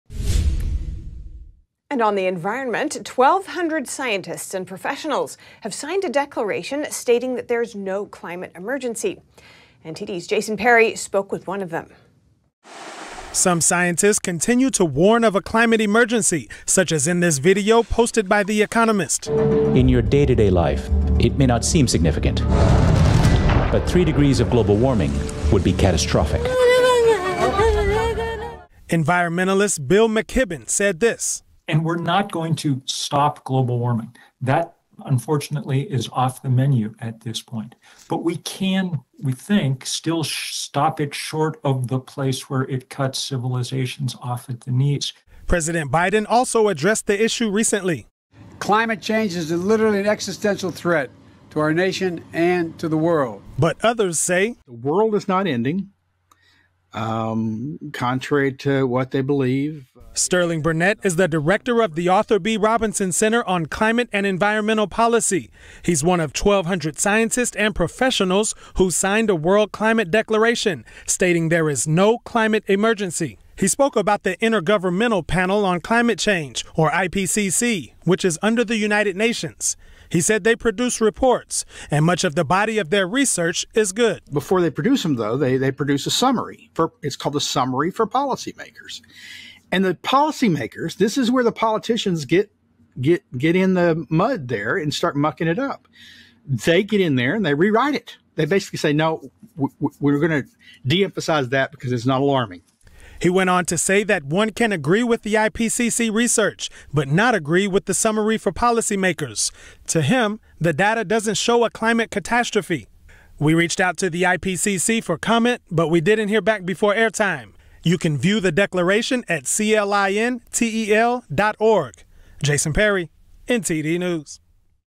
Scientists Sign Massive Declaration Admonishing Climate Alarmism
1,200 scientists and climate professionals have signed a declaration admonishing the narrative that the Earth is tumbling towards a climate change induced apocalypse. H. Sterling Burnett, director of The Heartland Institute's Arthur B. Robinson Center on Climate and Environmental Policy, joins NTD News to discuss his issues with the politicization of climate science.

Support more content like this by giving a tax-deductible donation to The Heartland Institute.